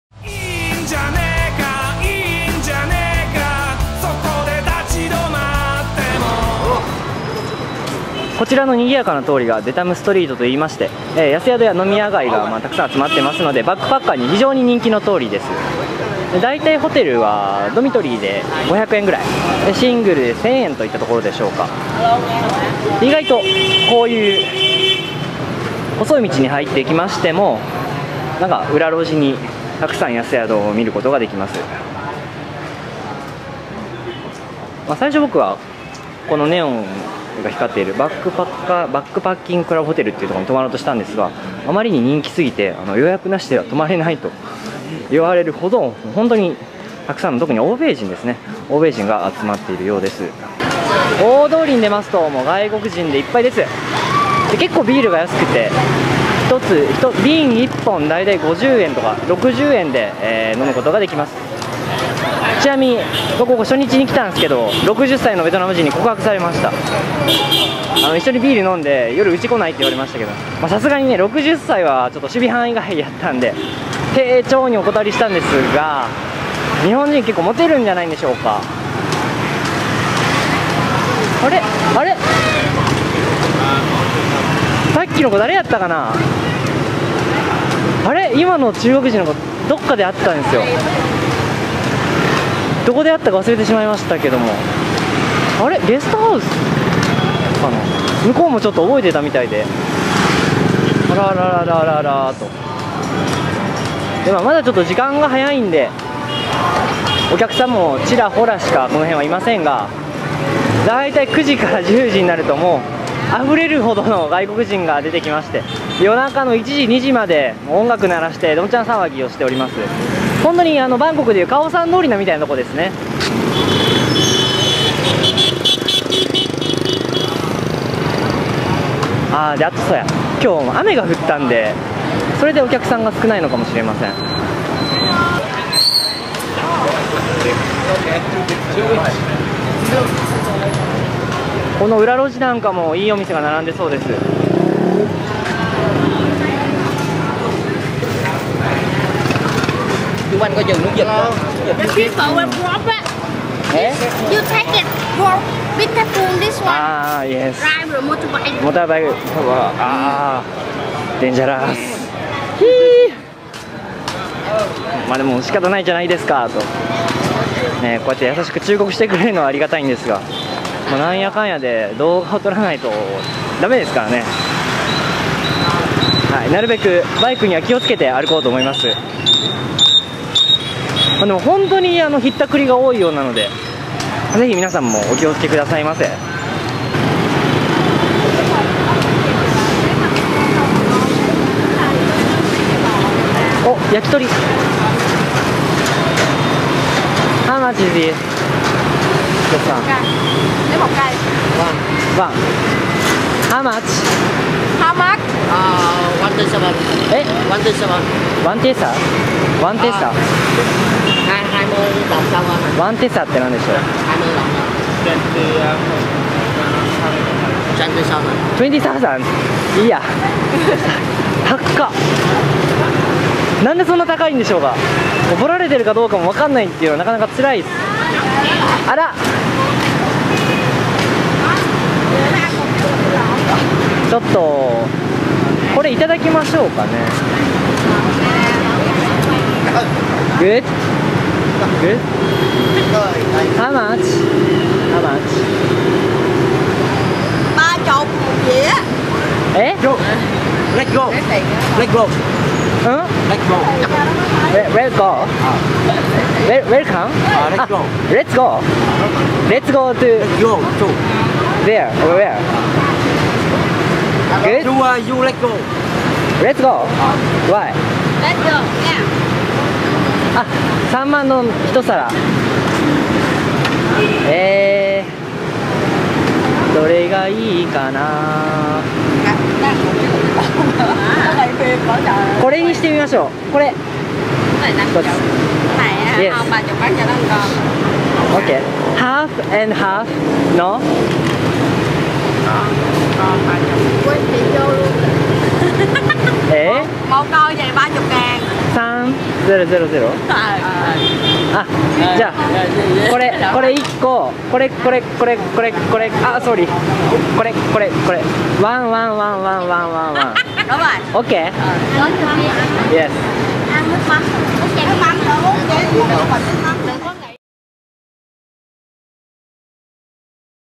デレッチョ ベトナム編 第116話 ホーチミンぶらぶら 夜のお散歩2
デレッチョ第3部ベトナム編　第116話　ホーチミン夜のお散歩の続きはバックパッカー街のデタム通りの紹介！でもベトナム最終日という事でせっかくなので今まで旅行中に食べた事無いものを食べようとローカルの方に戻ってきました。